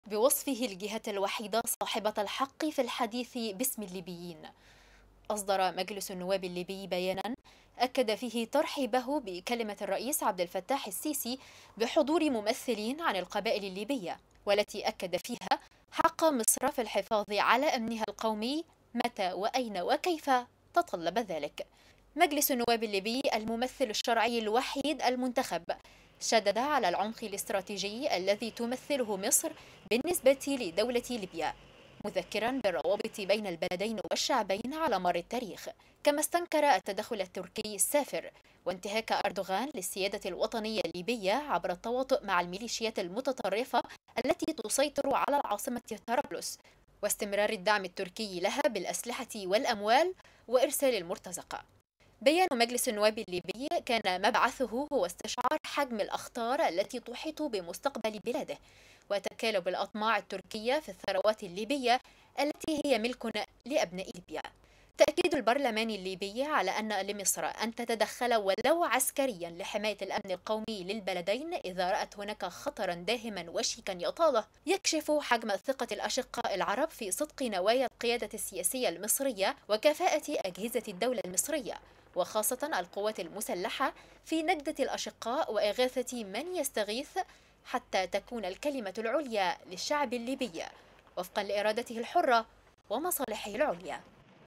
غرفة الأخبار | مجلس النواب الليبي يعرب عن ترحيبه بموقف الرئيس السيسي
17 يوليو 2020